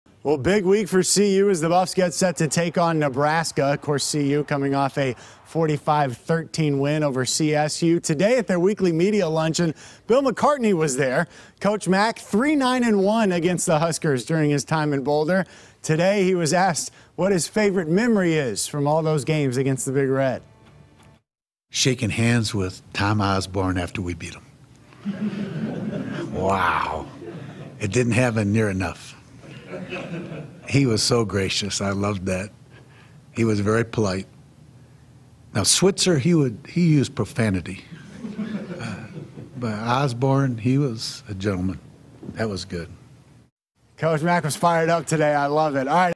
Coach Mac Visits Media Lunch Ahead Of CU-Nebraska Matchup
On Tuesday, former Coach Bill McCartney was at the team's weekly media lunch.